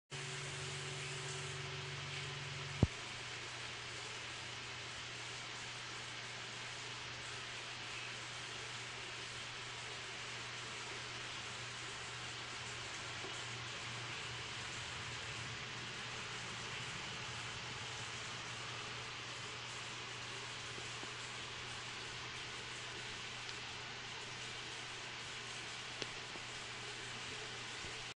Select Butterfly Koi 6-8 inches Bergen Water Gardens
On June 27, 2014 we received a nice selection of Select 6 to 8 inch Butterfly Koi These are an excellent medium size koi to add to your pond, bergenwatergardens,com